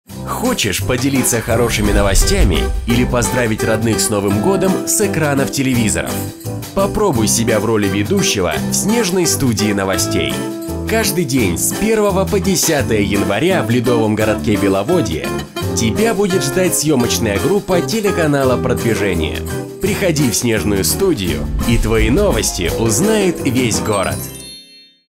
Снежная студия новостей
С 1-го января каждый, кто побывал в сказке, может передать привет горожанам в эфире телеканала ❄ ❅ Прокатитесь на метро, сфотографируетесь у статуи Егора Летова и попробуете себя в роли ведущего на TV❆ Поздравьте родных и друзей с Новый Годом!

До открытия ледового городка "Беловодье" осталось всего 2 дня  ☃